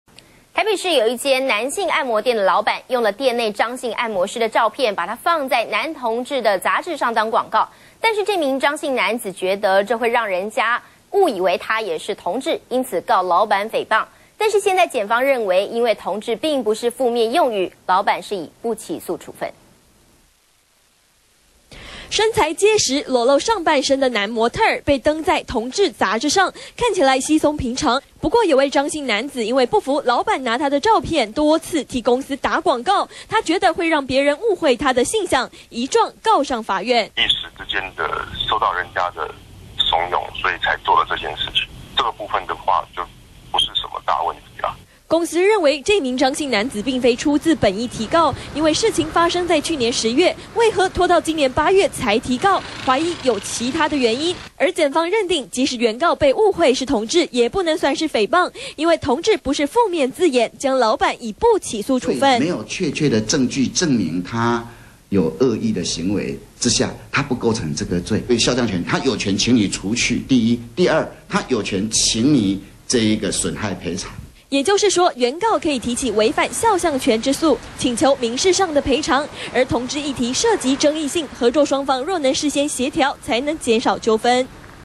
照片變同志按摩店廣告 控誹謗不成
中視新聞》台北一名張姓按摩師原本在一家男性SPA按摩店任職，不過該店負責人曾徵求張姓按摩師同意拍攝照片，刊登在《同歡指南》，事後張姓按摩師發現照片同時登上同志手冊，張姓按摩師憤而控告負責人涉犯加重誹謗罪。